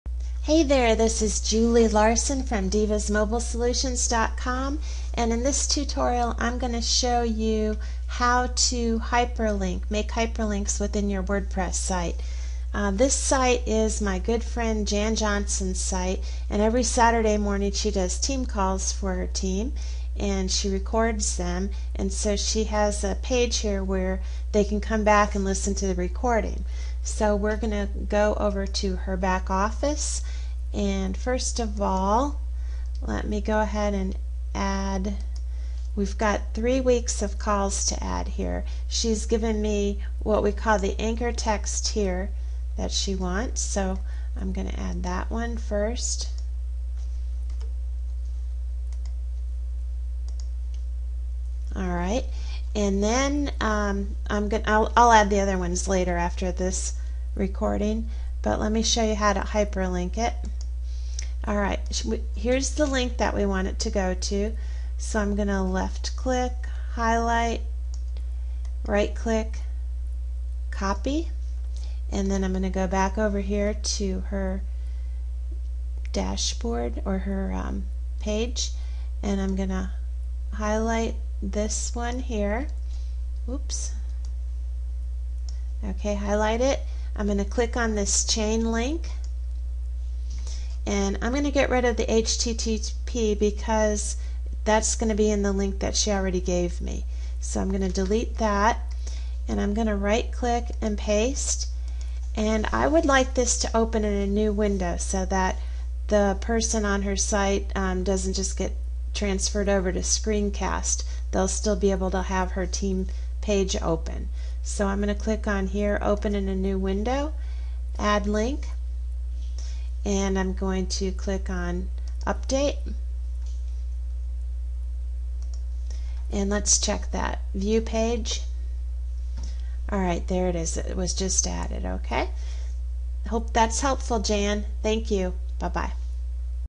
Adding a Hyperlink in WordPress
- Just like anything, adding a hyperlink in WordPress is easy once you know how!

In this tutorial, I'm showing my friend Jan Johnson how to add a hyperlink that leads to a recorded call for her RestartLeaders team site.

For more WordPress help and marketing tips, and sign up for our newsletter!